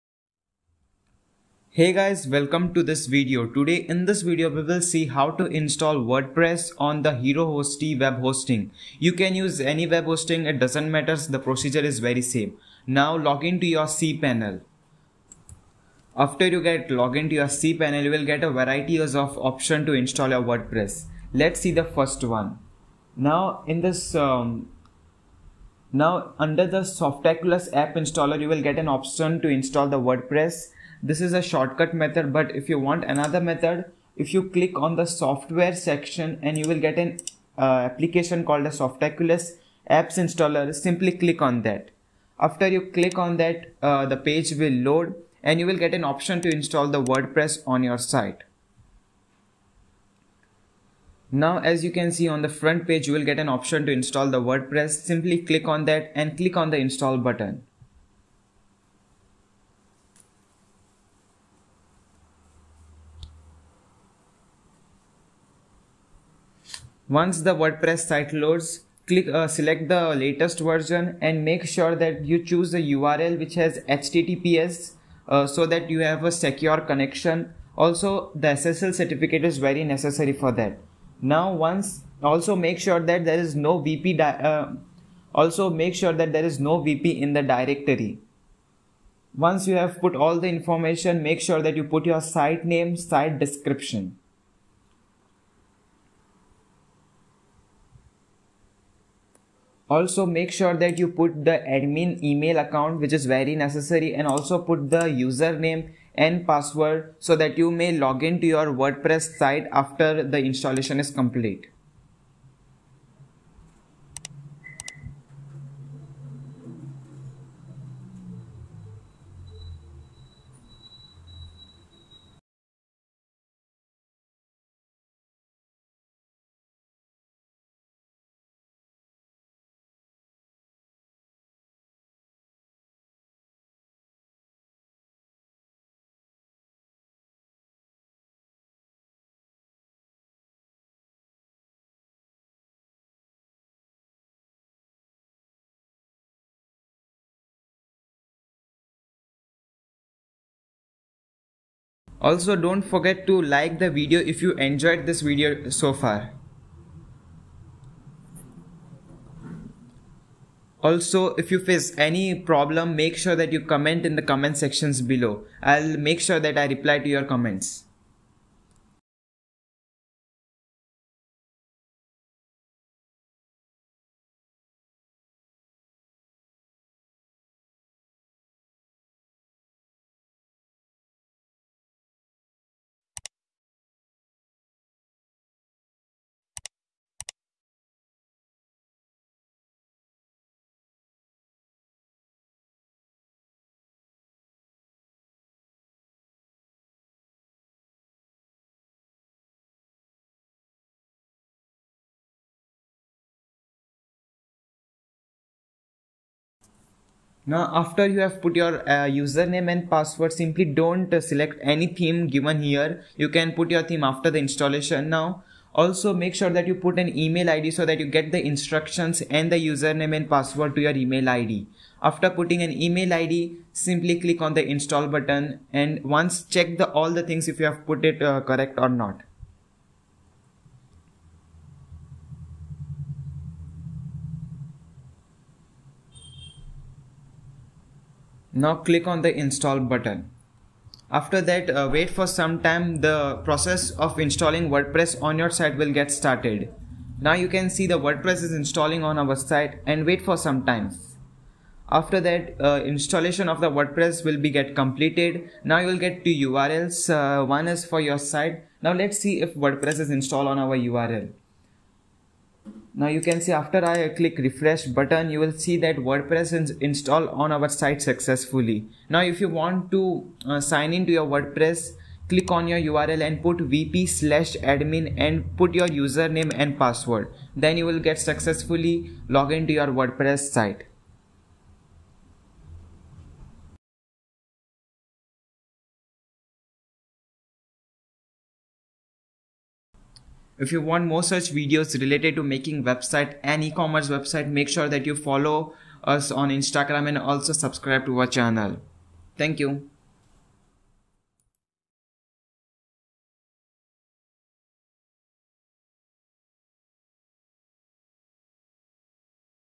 How to Install WordPress on Herohosty web hosting 2020
In this video we have shown how to install WordPress on herohosty web hosting
using simple steps